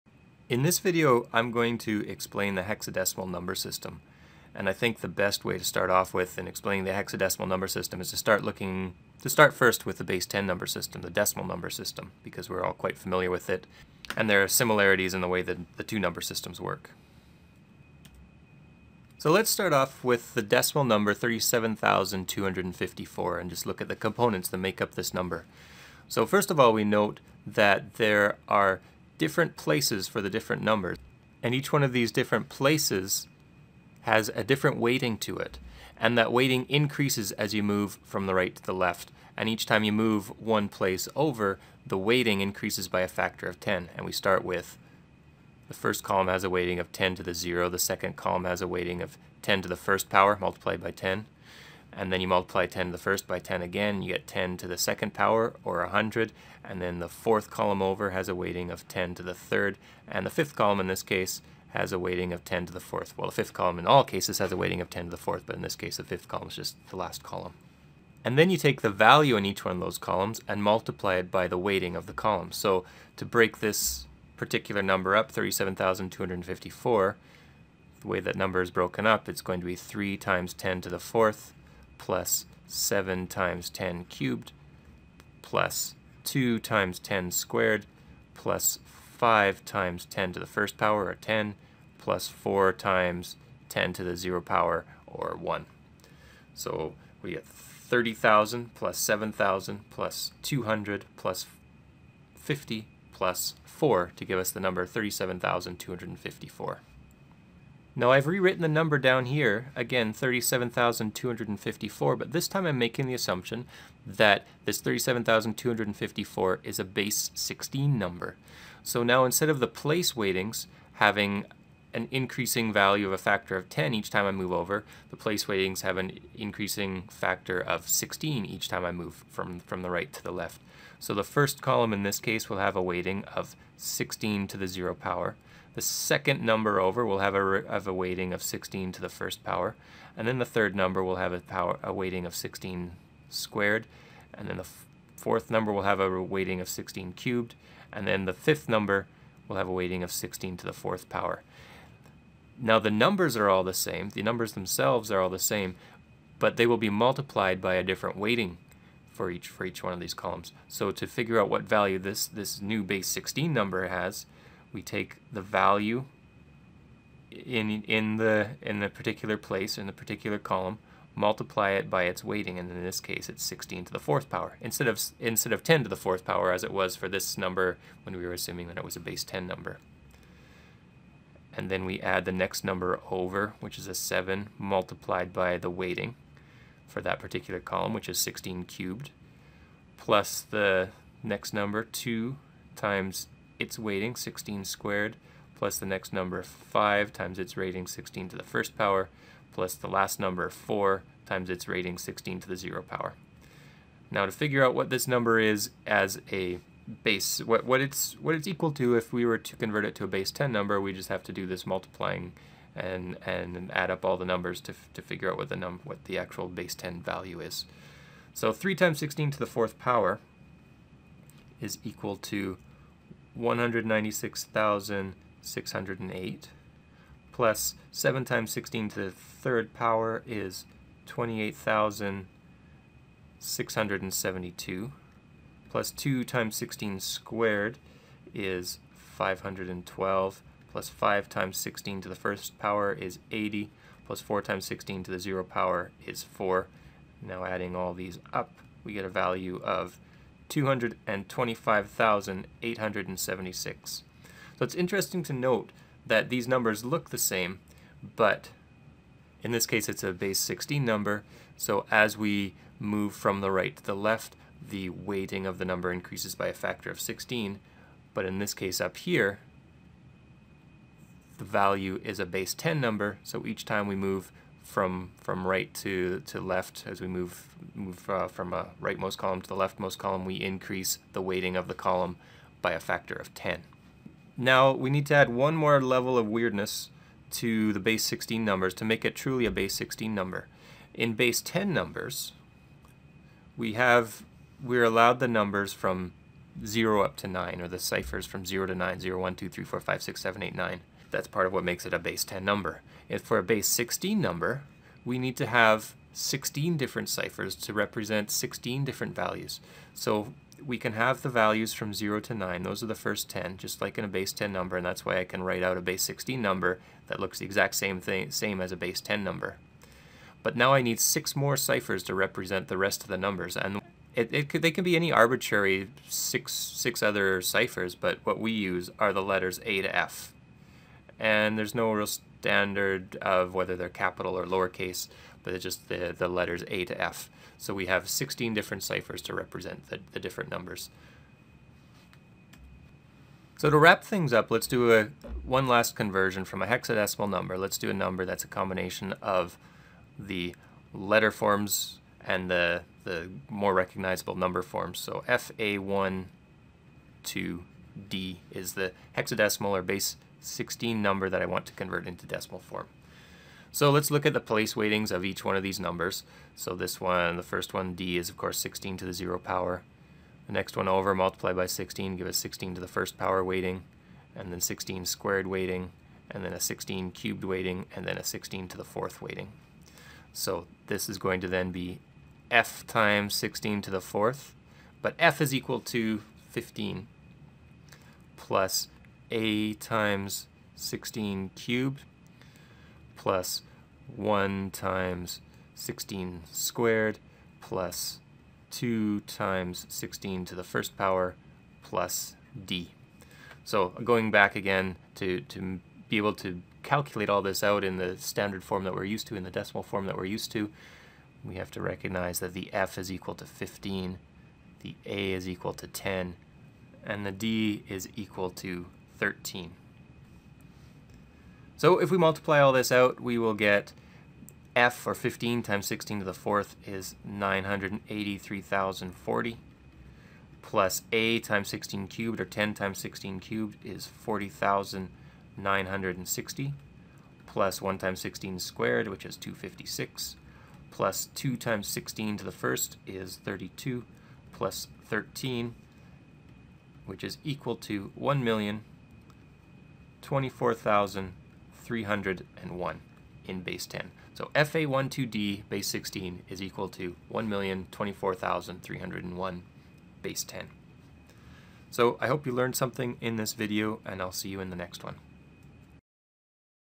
Hexadecimal Numbers
An introduction to hexadecimal numbers, this video compares the hexadecimal number system to the decimal number system and shows how to convert a hexadecimal number to a decimal number